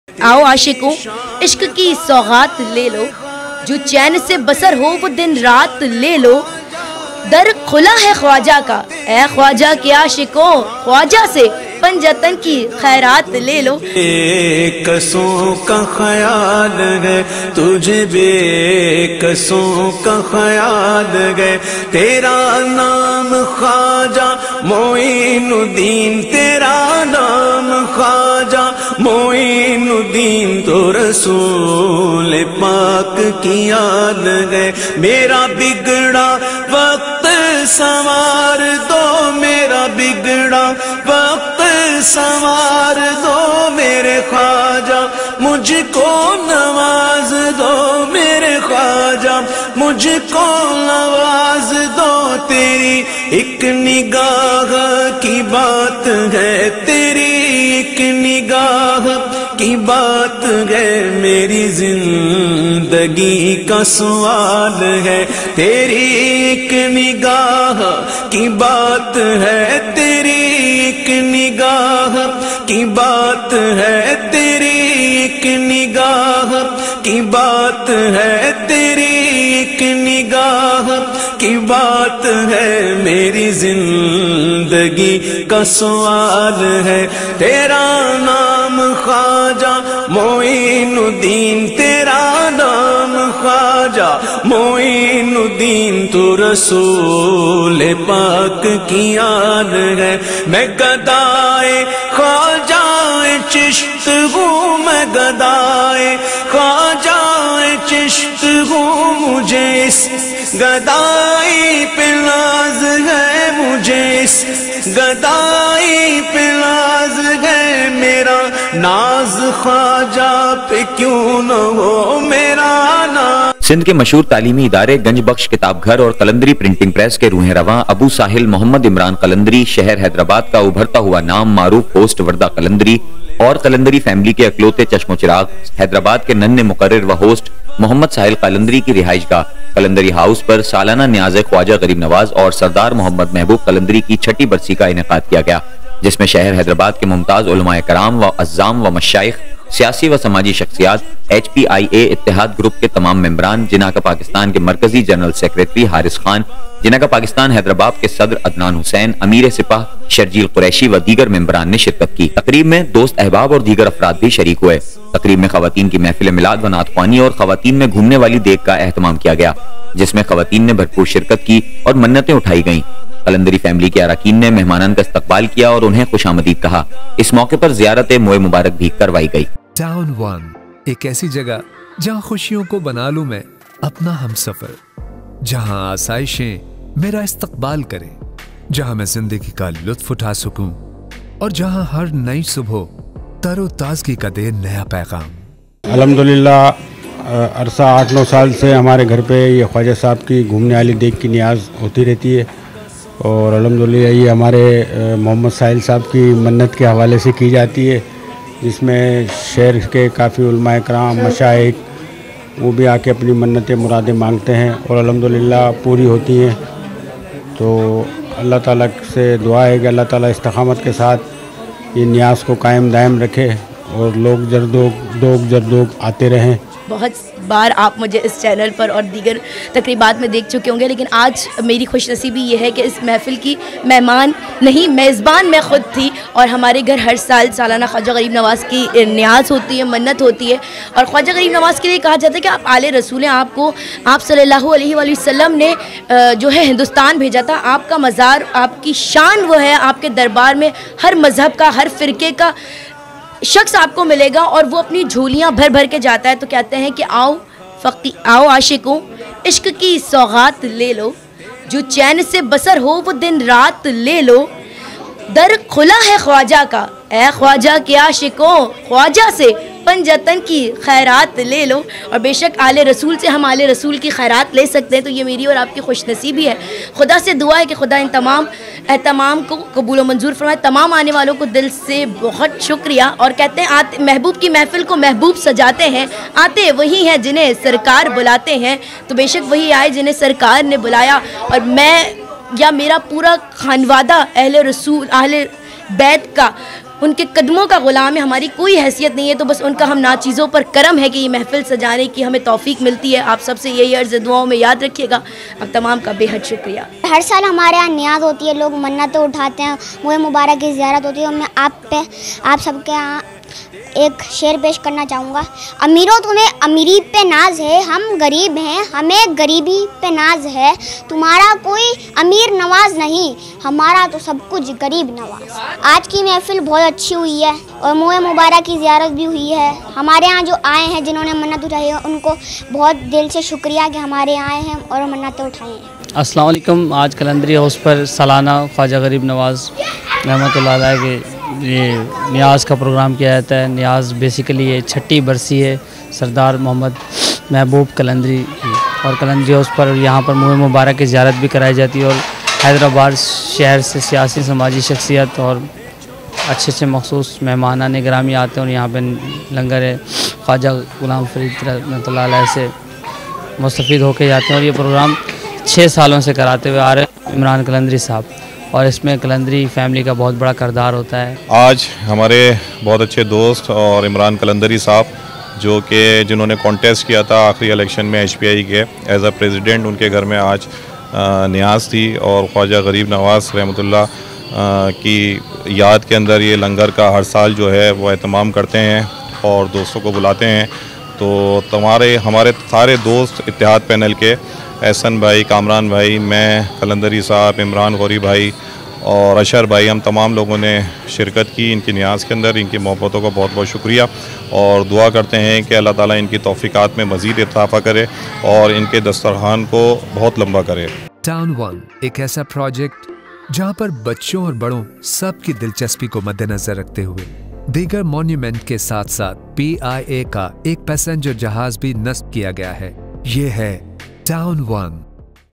Qalandari House Niaz
قلندری فیملی کی جانب سے سالانہ نیاز خواجہ غریب نواز کا انعقاد کیا گیا